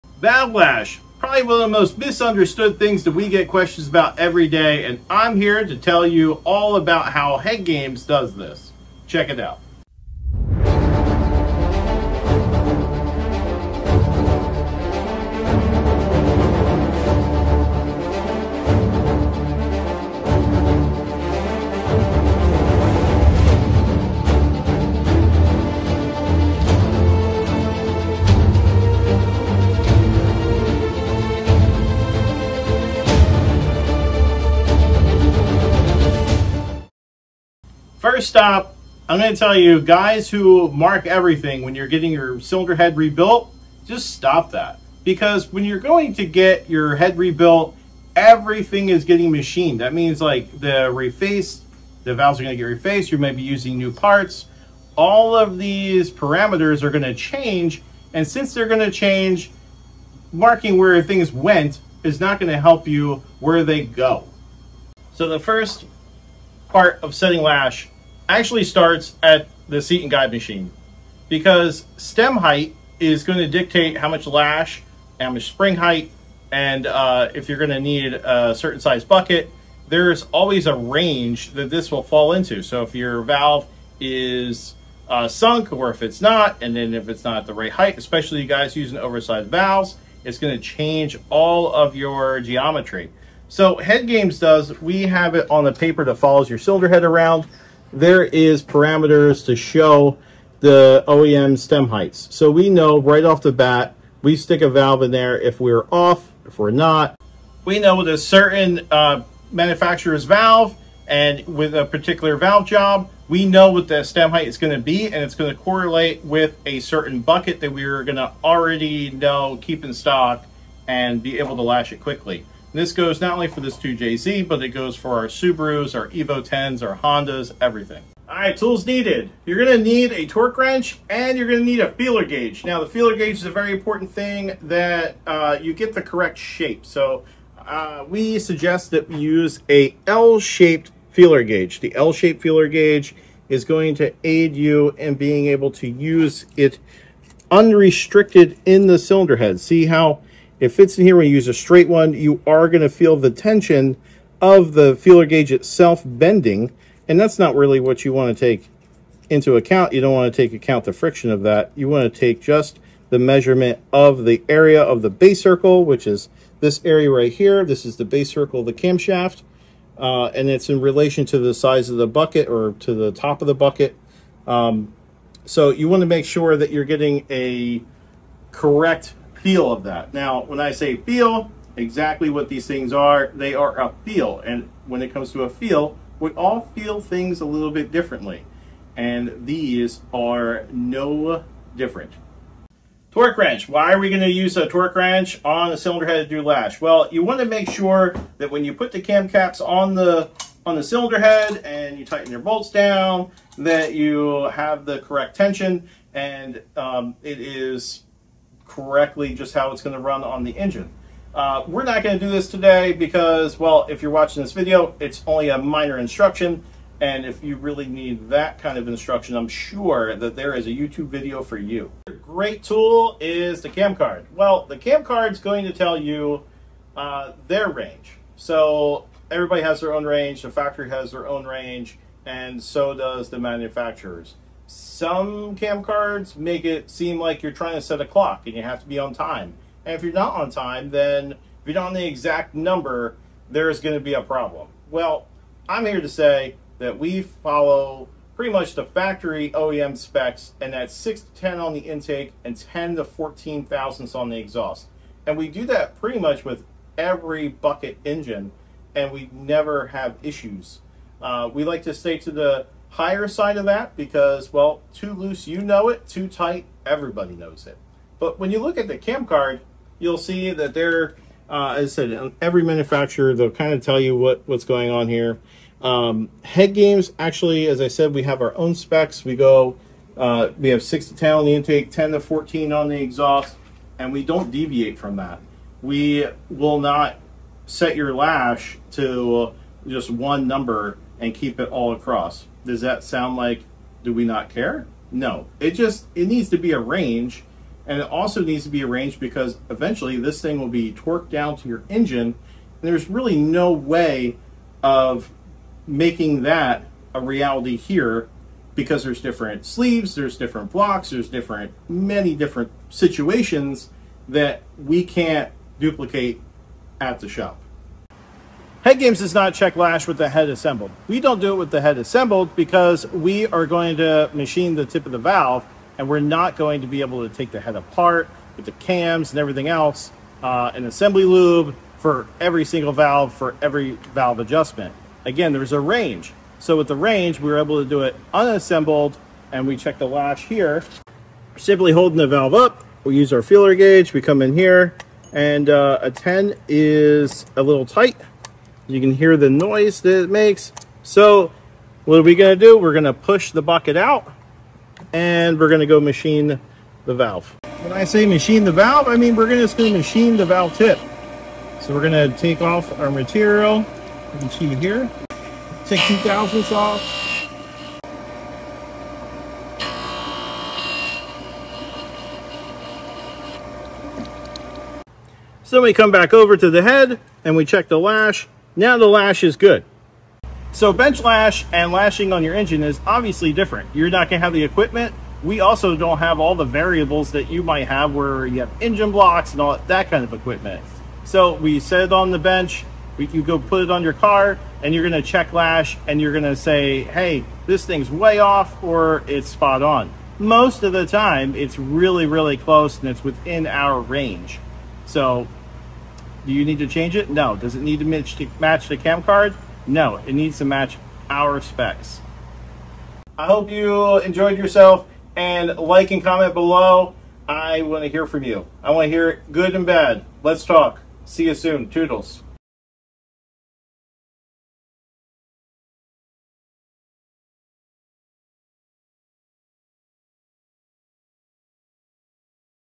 How do we set lash adjustment with solid buckets?
When you are a machine shop, you don't exchange buckets till you get the lash you need. You machine the valve or the bucket! HeadGames approach is simple, we have the stem height correspond to the spring height we desire along with the bucket we would like to use. Then we minimally tip the valve to achieve our lash. And we do this within a range of .004 (4 thousandths) because you are not setting a clock! Follow along as we show you how we have been doing this for over 20 years with success!
Remember with lash :To loose you know it, too tight everyone will know it.